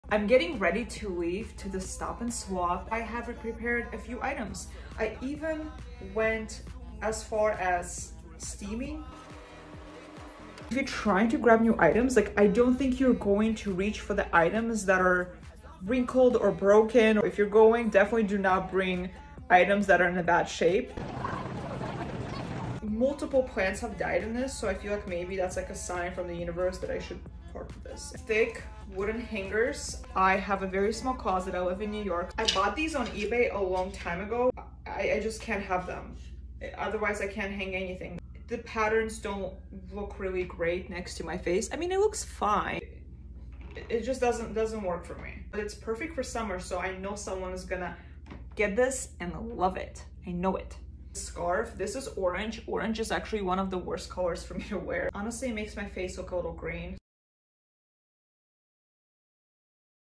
GrowNYC ♻️ Stop 'N' SwapEvent in NYC Behind the Scenes | INGA
This swap is over (check out behind the scenes!) but they have a few more coming up in Bronx, Queens, Brooklyn, and Manhattan! This is a great opportunity for NYers to find what they are looking for and part with preloved items!